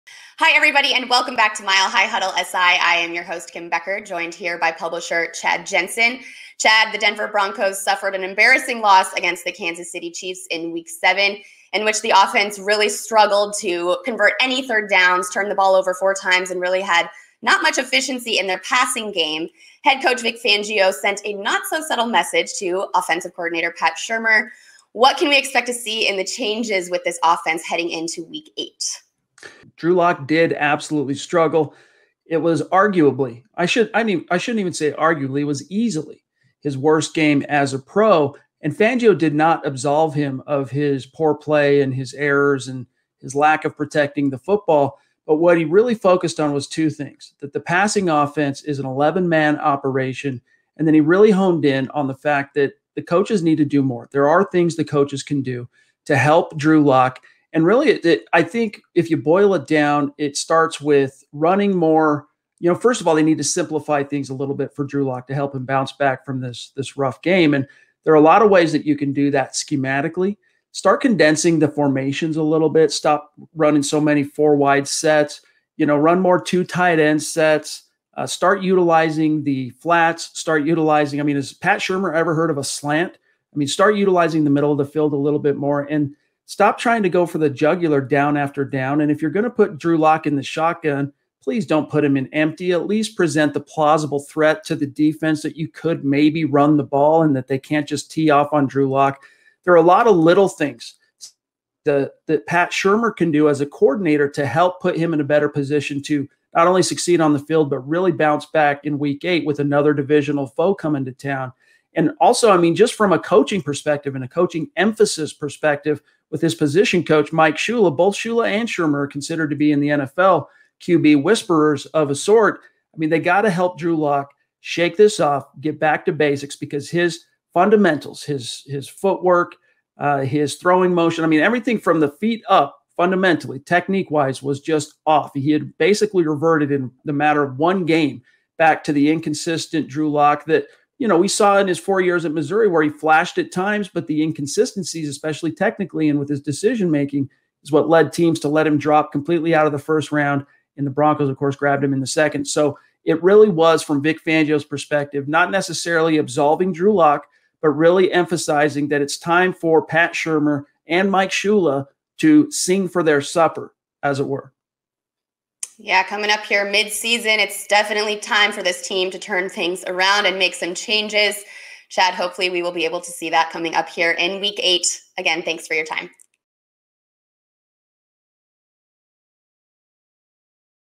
Fangio Turns Up Heat on Pat Shurmur: Fix the Offense | SI Sitdown
Chad Jensen joins Kim Becker to analyze what Vic Fangio had to say about the Broncos offense and interpreting his message to both OC Pat Shurmur and Drew Lock.